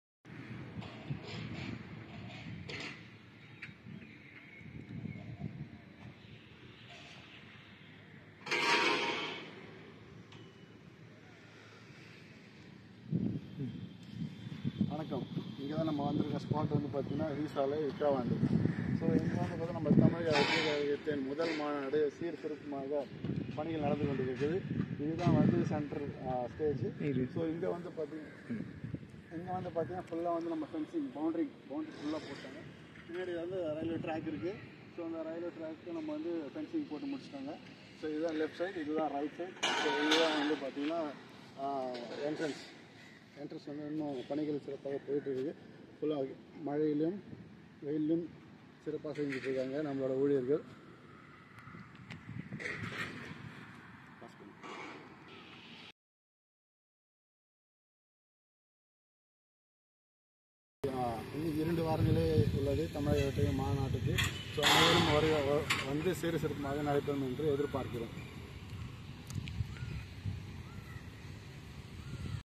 TVK Maanadu | த.வெ.க மாநாடு | V salai| Villupuram
த.வெ.க மாநாடு திடல் தற்போதைய நிலை.